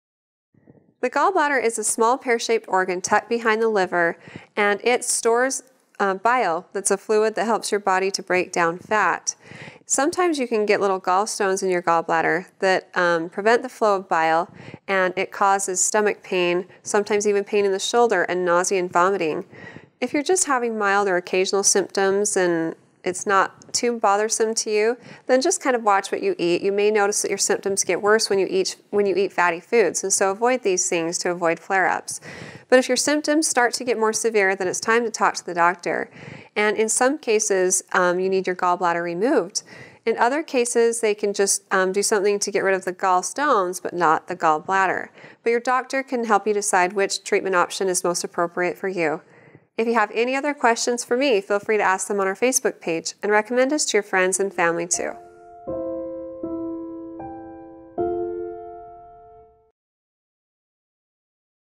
What can I do for gallbladder flare-ups?
The gallbladder is a small pear shaped organ tucked behind the liver and it stores bile that's a fluid that helps your body to break down fat.  Sometimes you can get little gallstones in your gallbladder that prevent the flow of bile and it causes stomach pain, sometimes even pain in the shoulder, and nausea and vomiting.  If you're just having mild or occasional symptoms and it's not too bothersome to you then just kind of watch what you eat.  You may notice that your symptoms get worse when you eat fatty foods.  Avoid these things to avoid flare-ups.  But if your symptoms start to get more severe then it's time to talk to the doctor.  In some cases you need your gallbladder removed.  In other cases they can just do something to get rid of the gallstones but not the gallbladder.  You doctor can help you decide which treatment option is most appropriate for you.